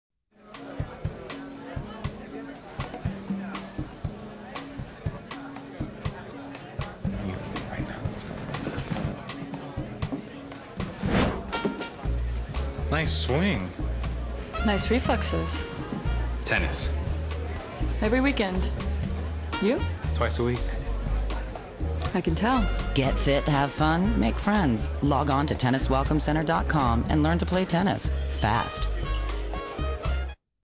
USTA "Forehand" Commercial
Vigilante casts tennis younger and hipper in this spot created for the USTA